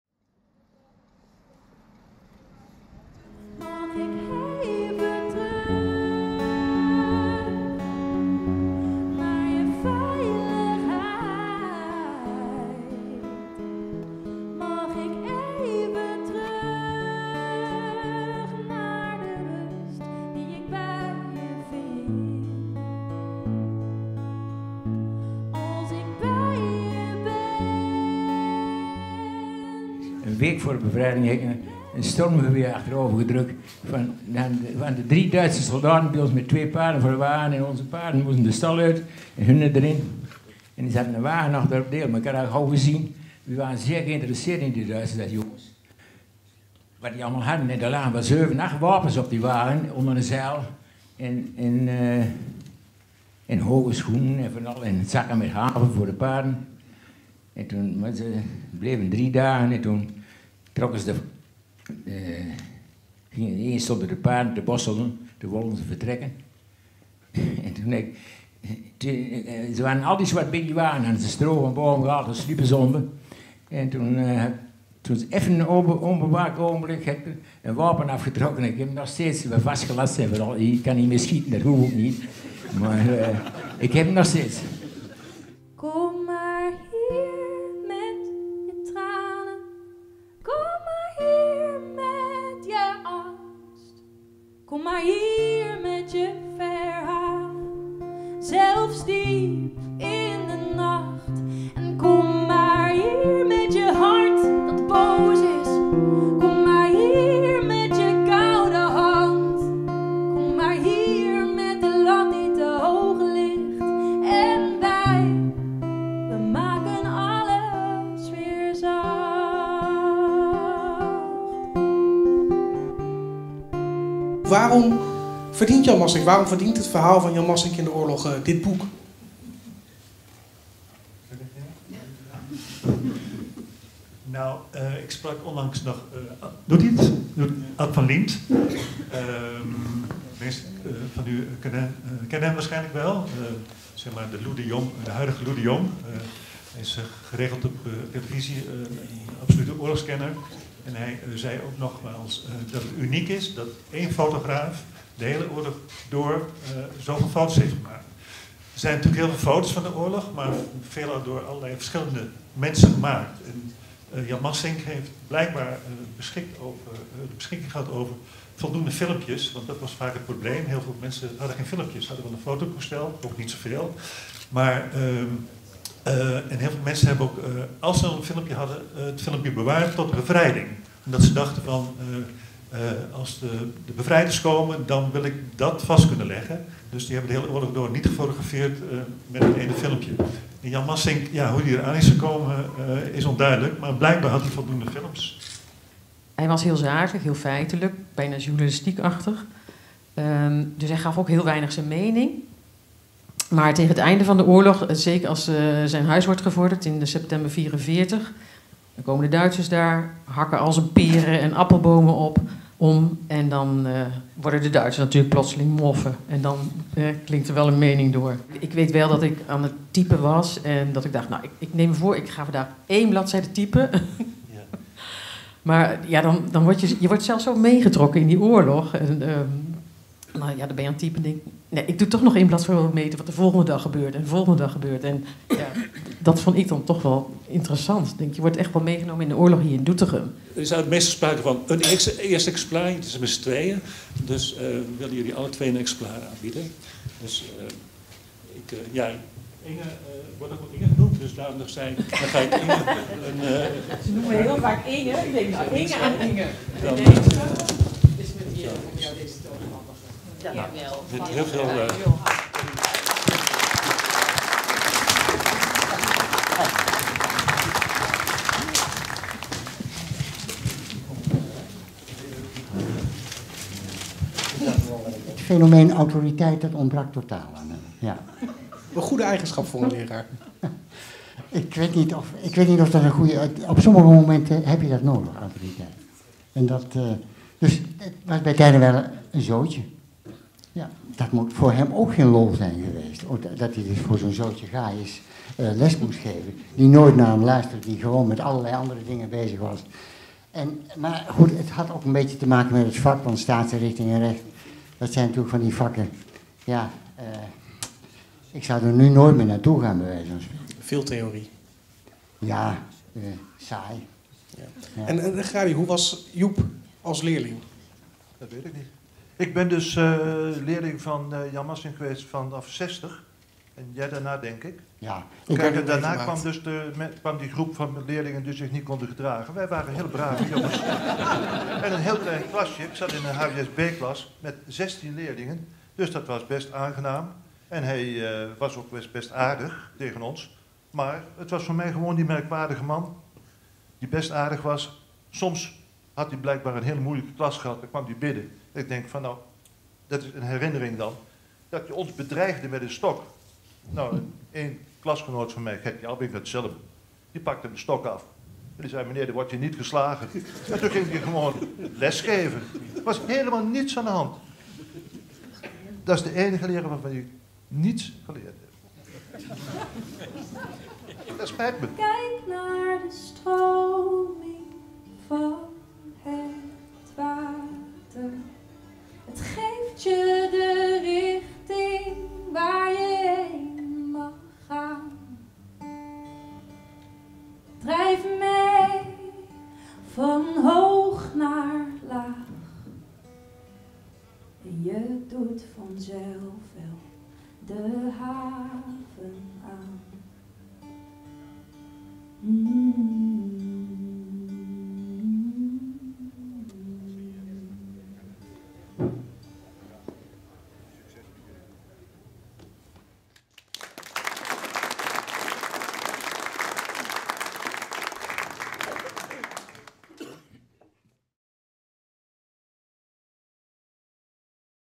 Boekpresentatie De oorlog van Jan Massink
Doetinchemmer Jan Massink (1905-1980) maakte tijdens de Tweede Wereldoorlog honderden foto’s.
Daar werden er 265 van uitgekozen voor het boek De oorlog van Jan Massink.
Op 21 november 2023 was de presentatie.